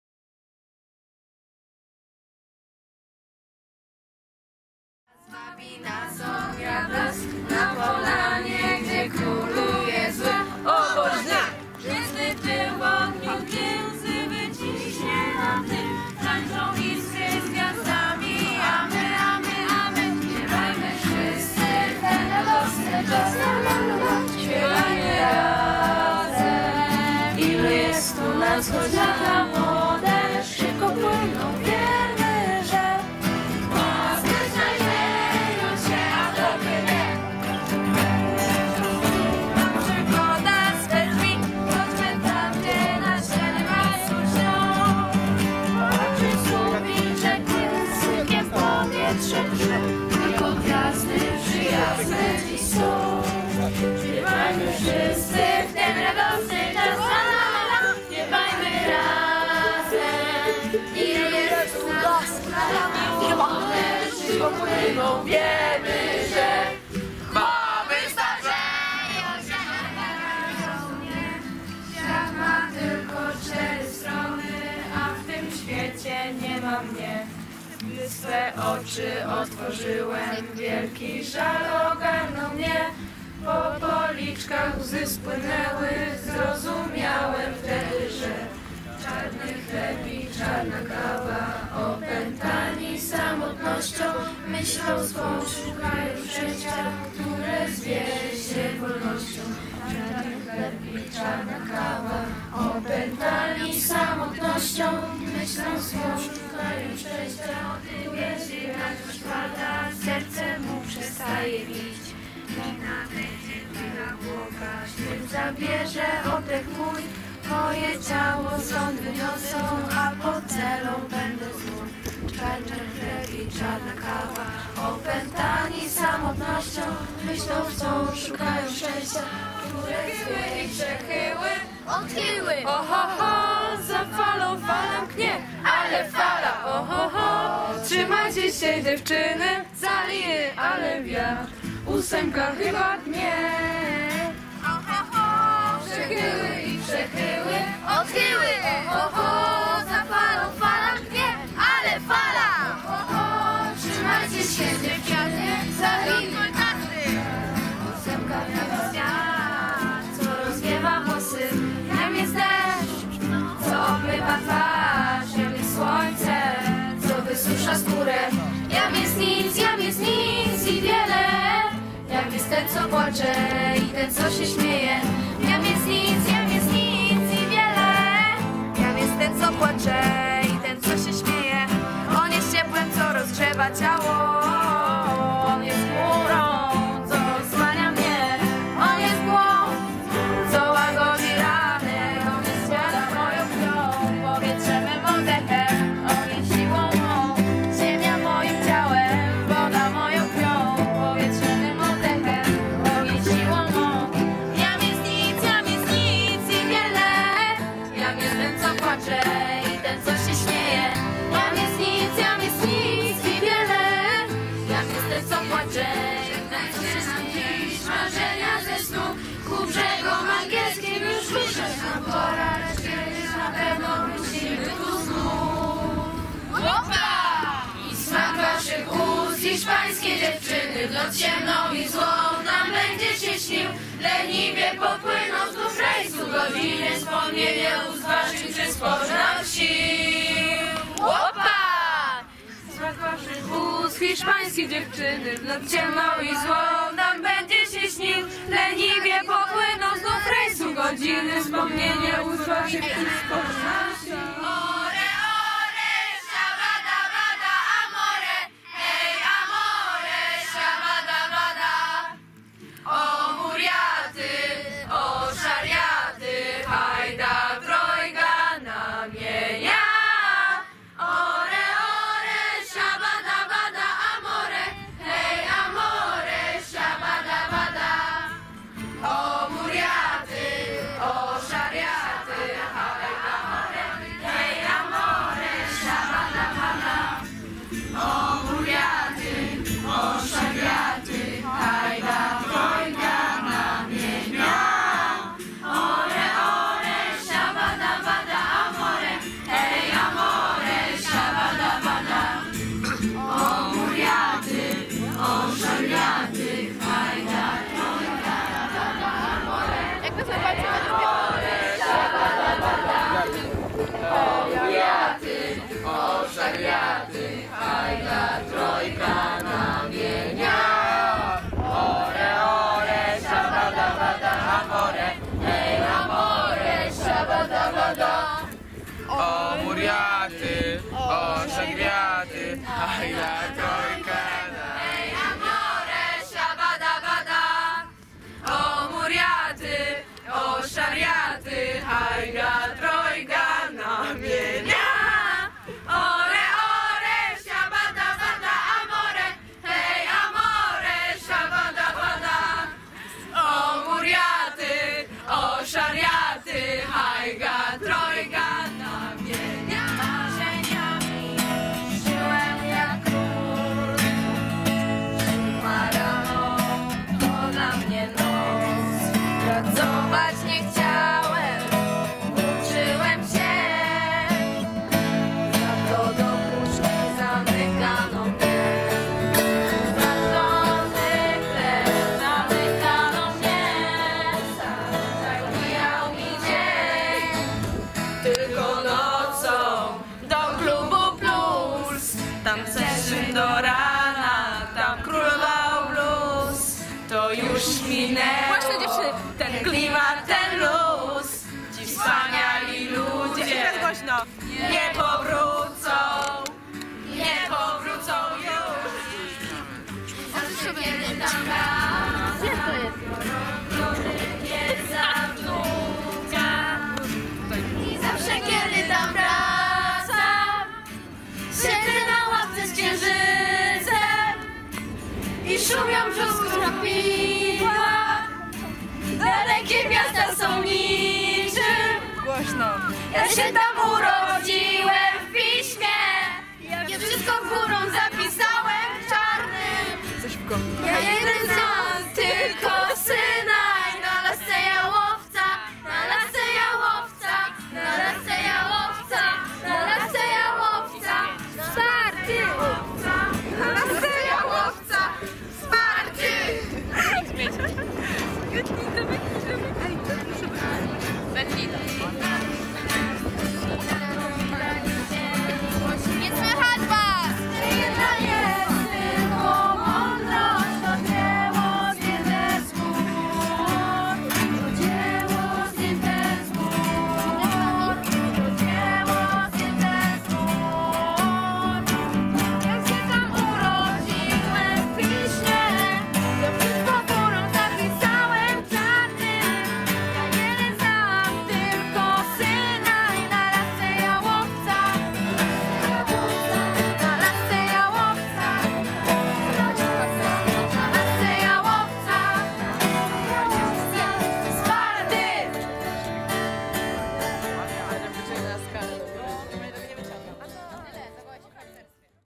Obóz Harcerski ZHP Mielec 2019. Śpiewanki przy ognisku, część 3. 22 lipca 2019
Od 13 do 28 lipca 2019 roku w Błotach Karwieńskich odbył się obóz Hufca ZHP Mielec. Śpiewanki to świetny sposób na wspólne spędzanie czasu. Śpiewajcie z nami!

Zdjęcia:  Maksymilian Gancarz, Maja Stelmach